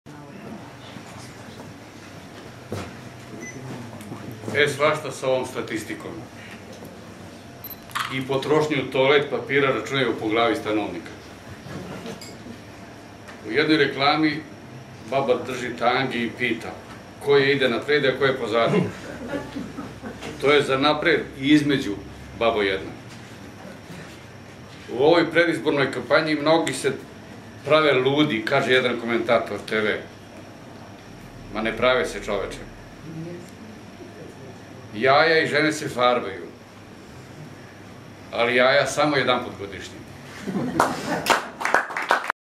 2012-11-12: Представљање зборника радова Изворник 3 (13. део)
У Смедеревској Паланци је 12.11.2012. у ресторану "ЛаПорта" у организацији Књижевног клуба 21 одржана је промоција 3 зборника радова Изворник У овом видео запису представљен је немонтиран материјал који је снимио Милан Милошевић у оквиру пројекта Паланка на вези